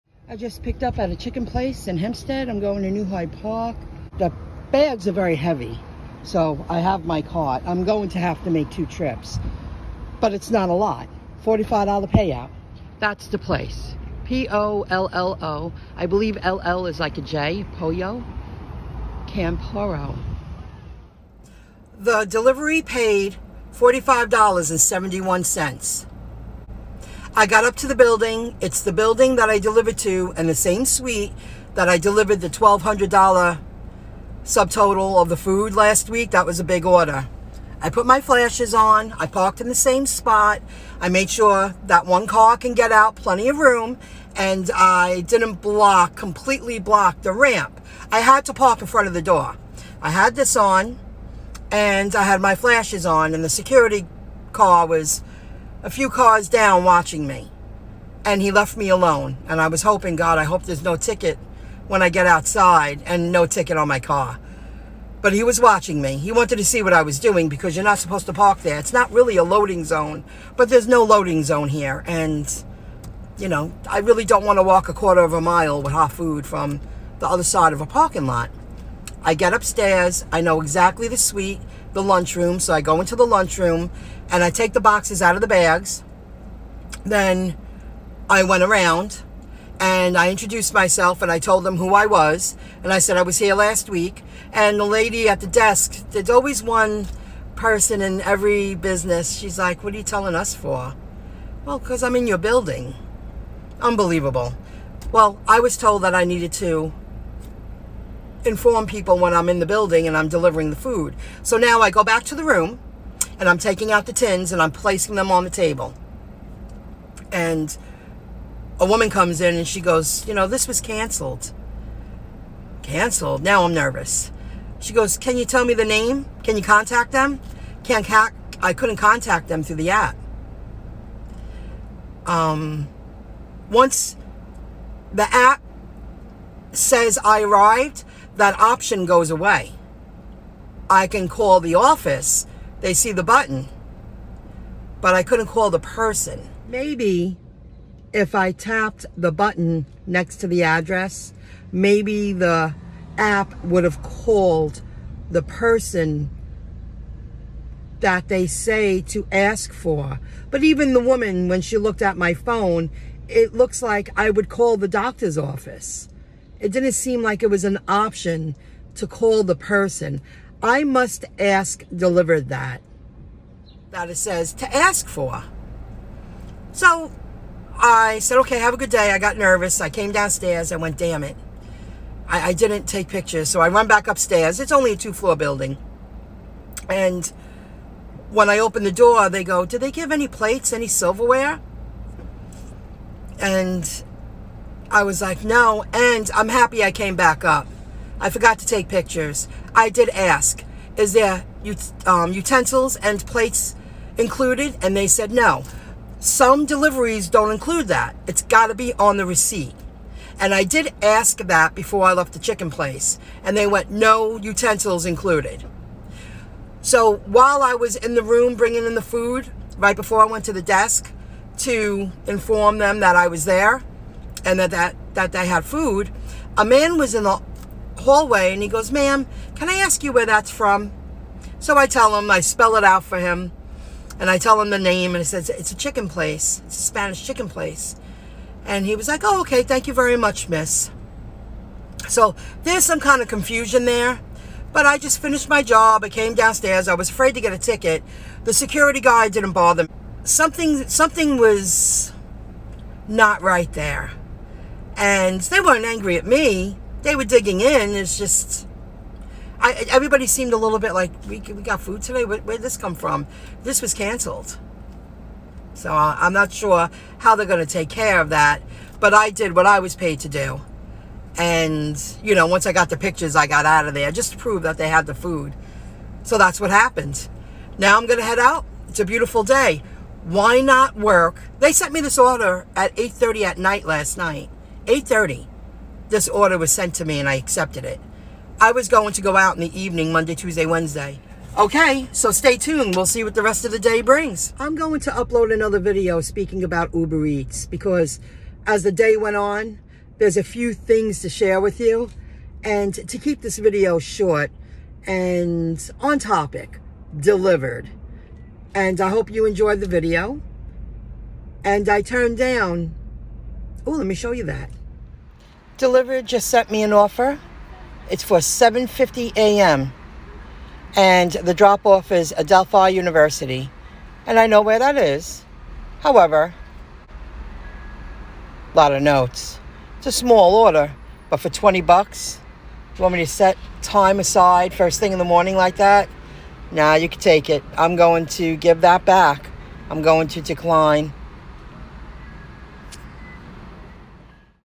I delivered a cancelled dlivrd delivery.
Order accepted 8:30 pm the night before.  Customer claimed order was canceled.

Soon Shop & Prepare jobs will soon get replaced by AI.  People will be less needed.